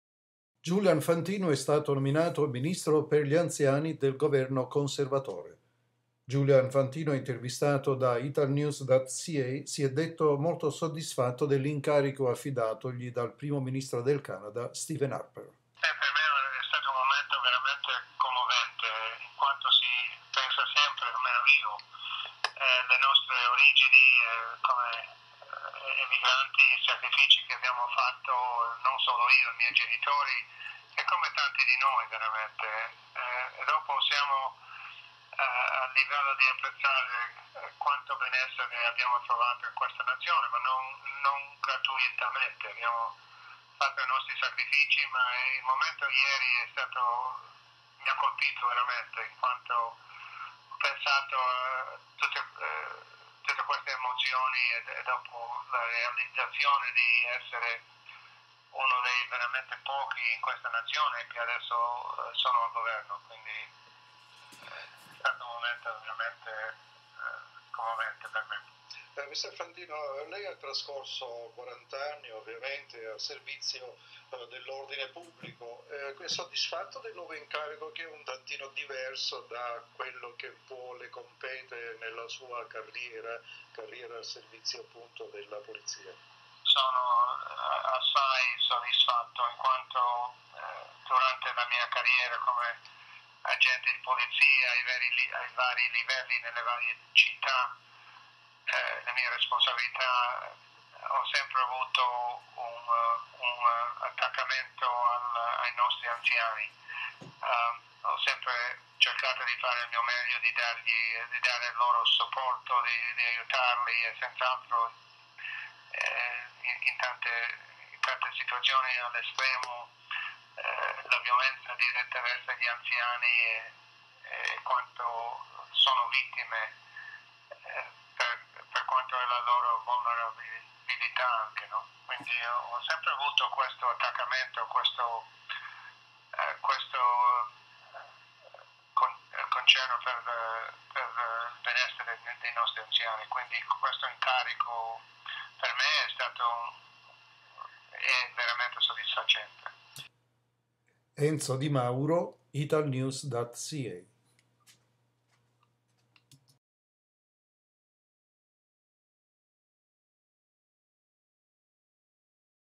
Julian Fantino Minister for Seniors.mov
Julian Fantino has been appointed Minister for Seniors of the Tory Government  by Prime Minister Stephen Harper. Fantino, elected to the House of Commons in the November by-election, is very satisfied with the position and is committed to be a strong voice for seniors of Vaughan and the whole Canada.